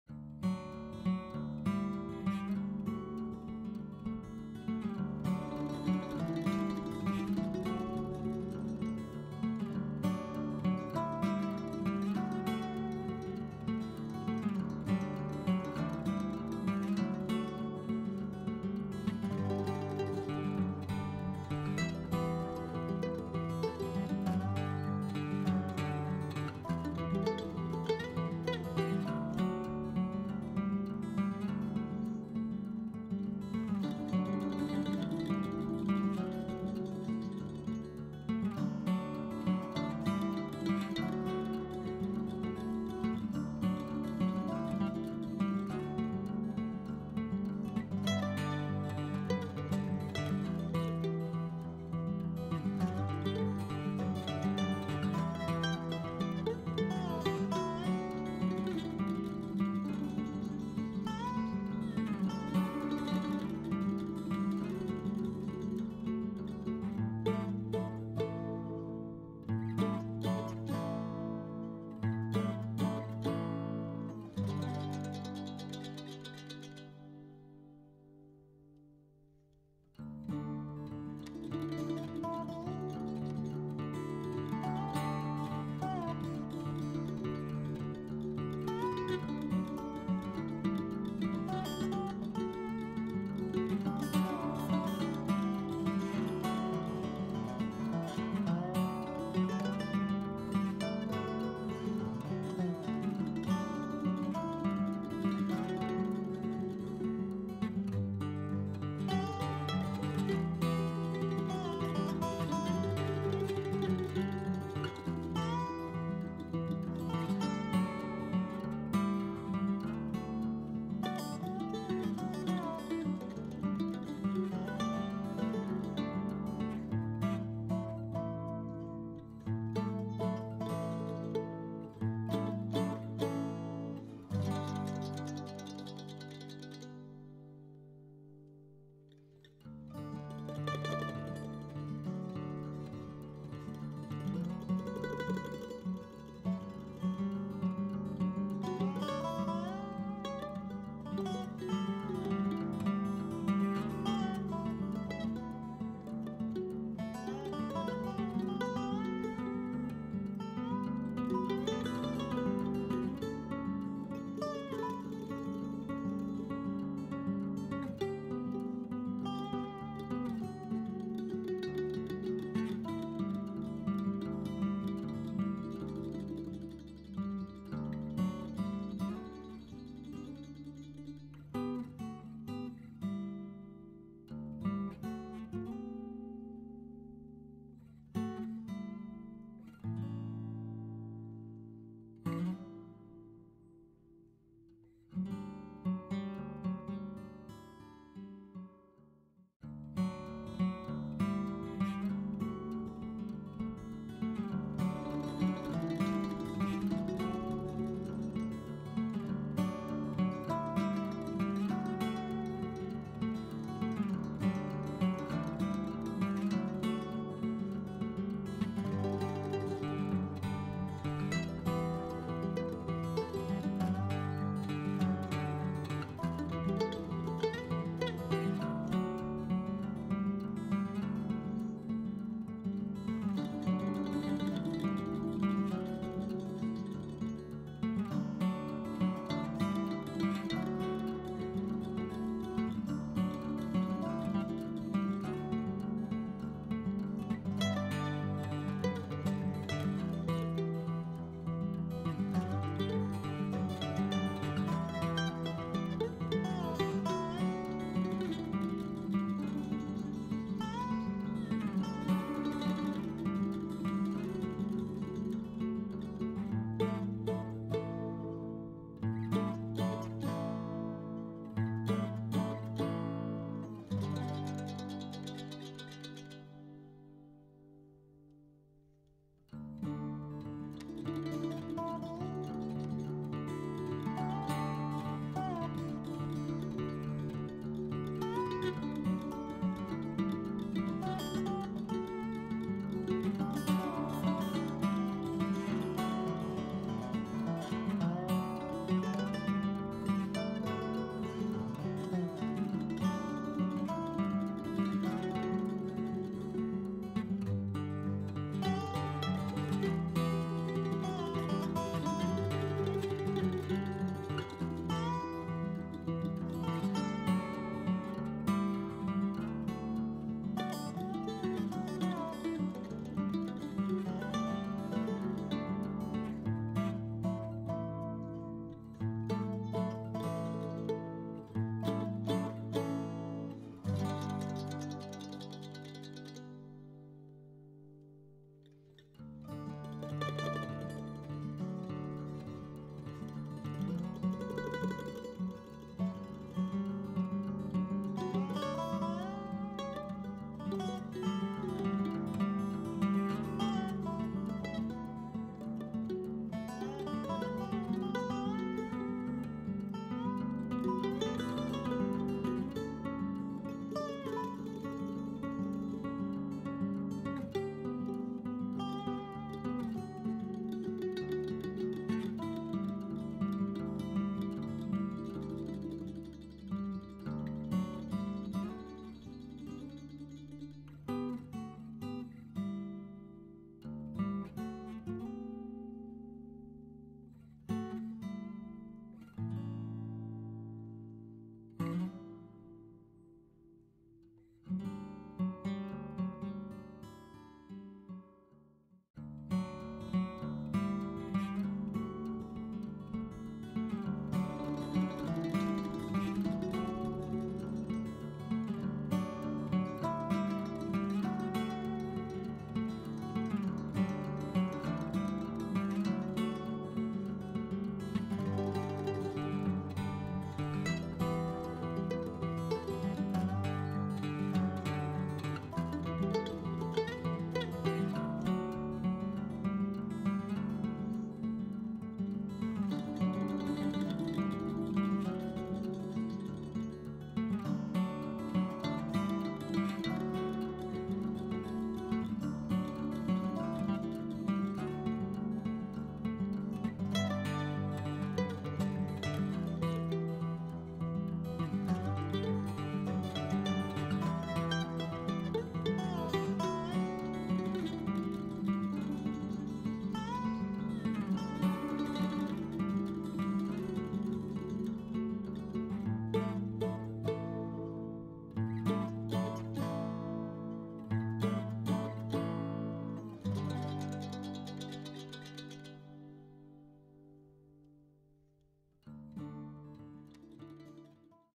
Great Idea with Women's Thin Socks and Lace ♻️ Look What I Did? 👍
Kullanılan Malzemeler
Dantel örtü
Ahşap kasnak
İnce çorap
Jüt kurdele ve ipi
Yarım inci